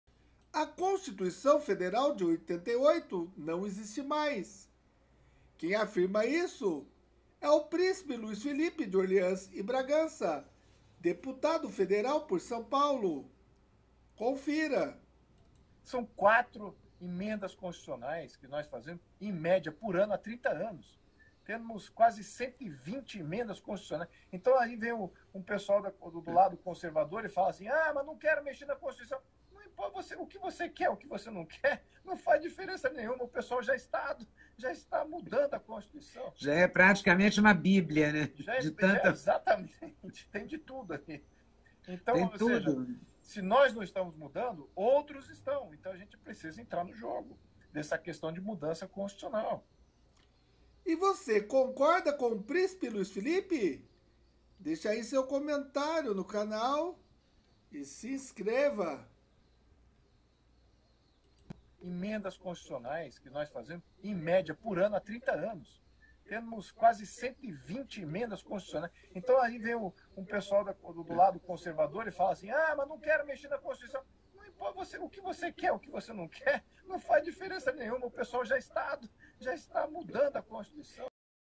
Não existe mais a Constituição de 88 - Príncipe Luiz Philippe de Orleans e Bragança
Não existe mais a Constituição de 88 - Príncipe Luiz Philippe de Orleans e Bragança . Ao invés de a sociedade liderar essas mudanças, elas estão ocorrendo da mesma maneira e sem a participação popular. Quando percebemos é tarde demais.
O caminho para frear essas mudanças é resgatar a soberania popular.